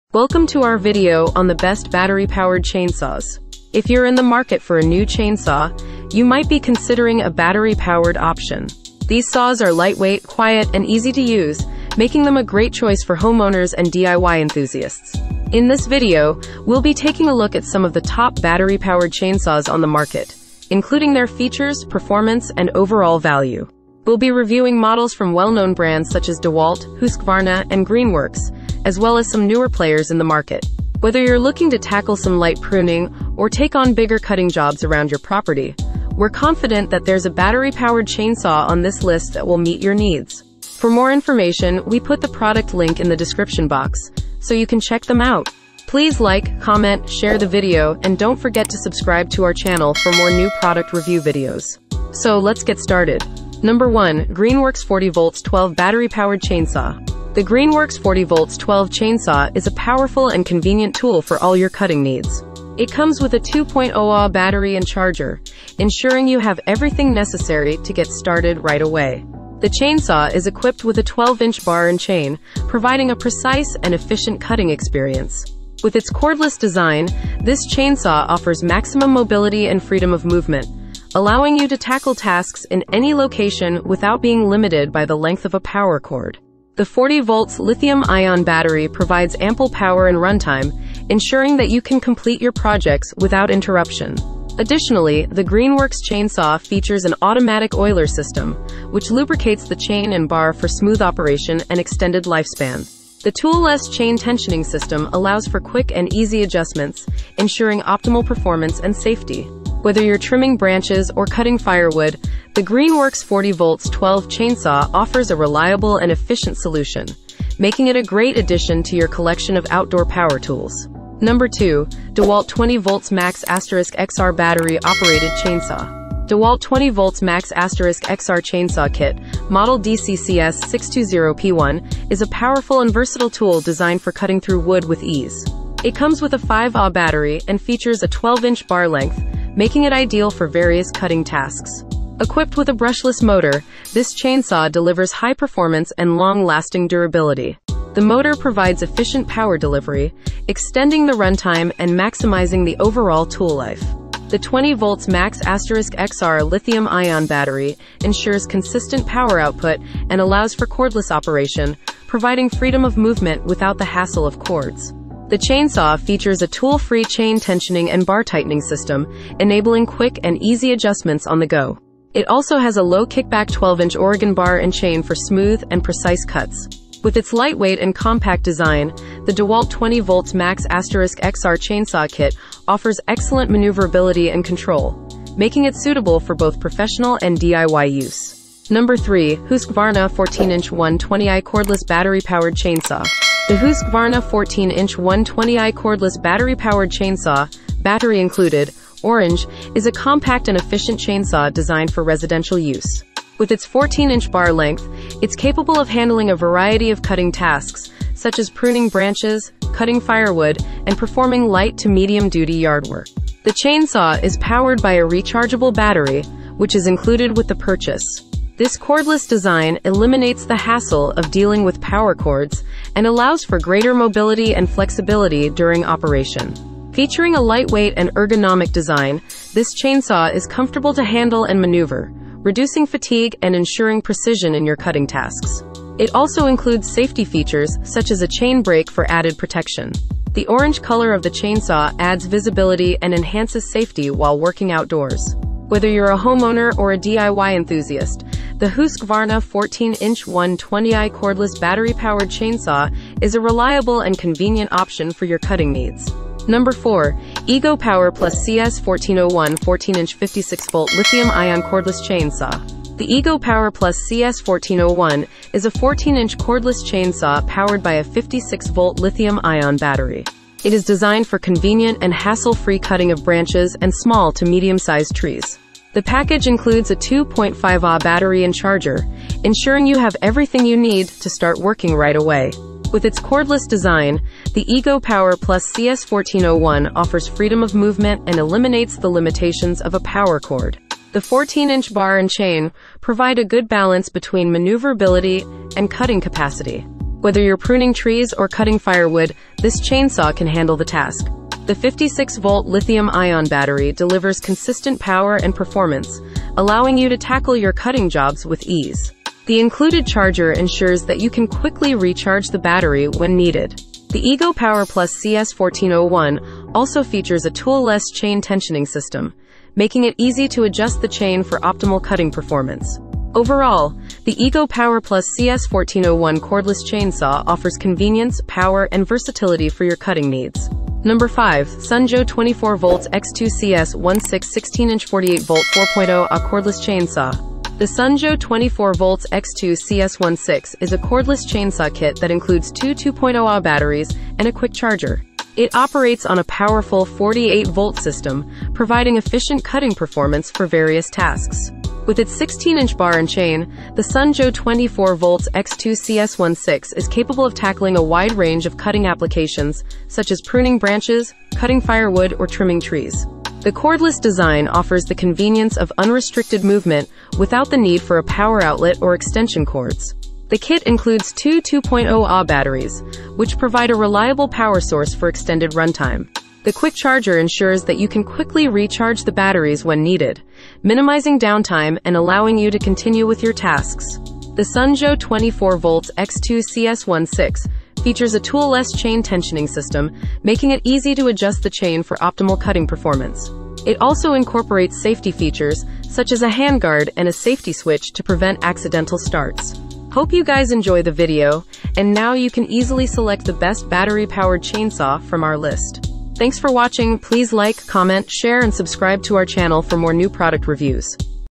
Best Battery Powered Chainsaw 2024 (Testing and Reviewing the Top 5 Options)
Best Battery Powered Chainsaw 2024 (Don't Buy One Before Watch This)

Number 1: Greenworks 40V 12" Battery Powered Chainsaw

Number 2: DEWALT 20V MAX* XR Battery Opereted Chainsaw

Number 3: Husqvarna 14 Inch 120i Cordless Battery Powered Chainsaw

Number 4: EGO Power+ CS1401 14-Inch Cordless Chain Saw

Number 5: Sun Joe 24V-X2-CS16 16-Inch 48-Volt Cordless Chain Saw

About This Video:

Welcome to our video on the best battery powered chainsaws! If you're in the market for a new chainsaw, you might be considering a battery powered option. These saws are lightweight, quiet, and easy to use, making them a great choice for homeowners and DIY enthusiasts.

In this video, we'll be taking a look at some of the top battery powered chainsaws on the market, including their features, performance, and overall value. We'll be reviewing models from well-known brands such as DeWalt, Husqvarna, and Greenworks, as well as some newer players in the market.

Whether you're looking to tackle some light pruning or take on bigger cutting jobs around your property, we're confident that there's a battery powered chainsaw on this list that will meet your needs.